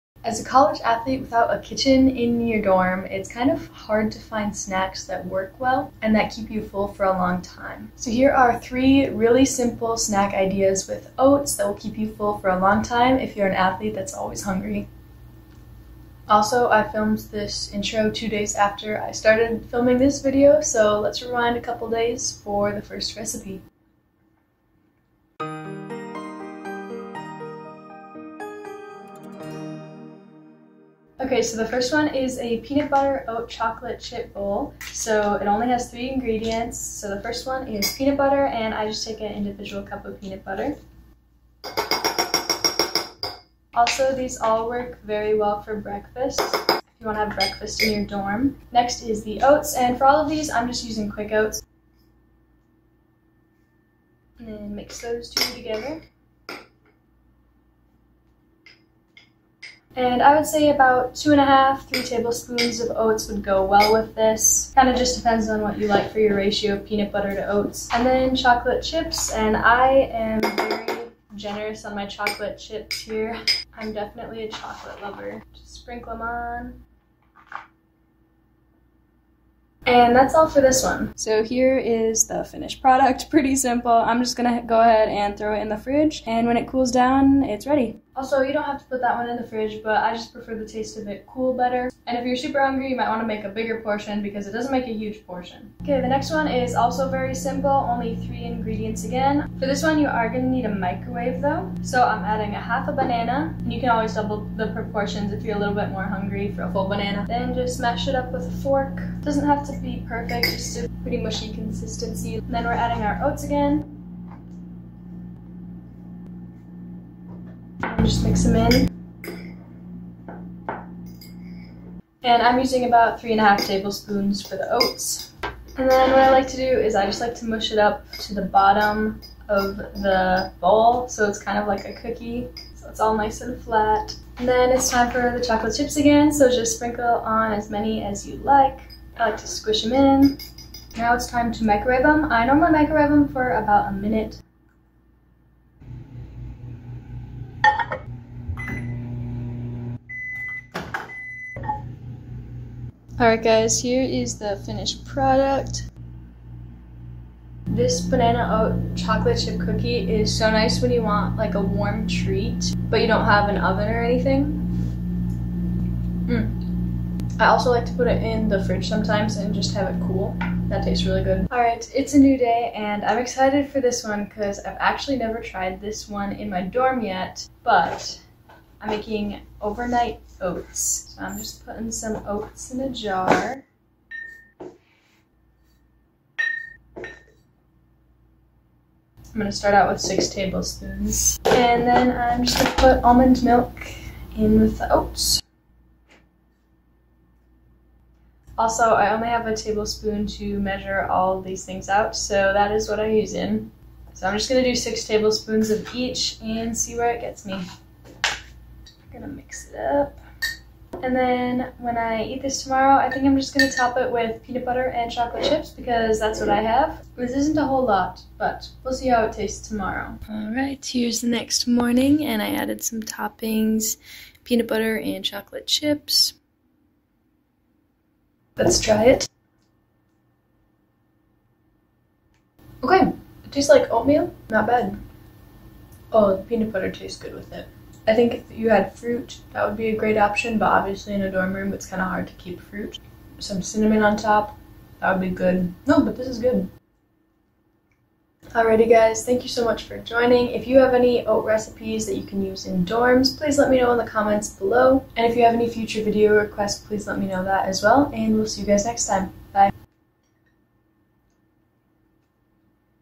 Snacks To Keep College Athletes Full - Oats Edition
If you are a college student and athlete who is always hungry, here are 3 quick and easy recipes to help you stay full for longer! No oven or stove is needed, only a microwave and fridge for different snacks!💜